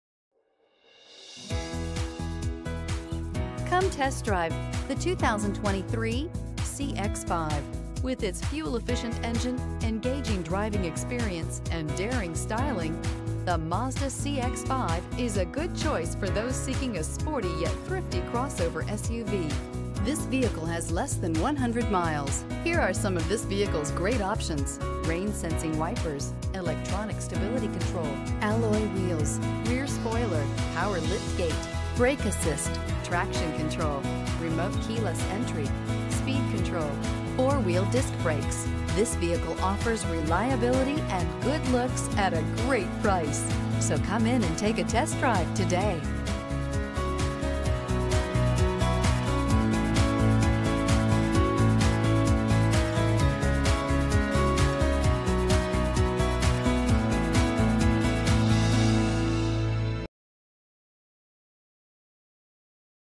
2023 Mazda CX-5 M14990
2023 Mazda CX-5 2.5 S Carbon Edition

Wilkins in Elmhurst
740 N York St

Polymetal Gray 2023 Mazda CX-5 2.5 S Carbon Edition AWD 6-Speed Automatic SKYACTIV 2.5L 4-Cylinder DOHC 16VbrbrRecent Arrival! 24/30 City/Highway MPG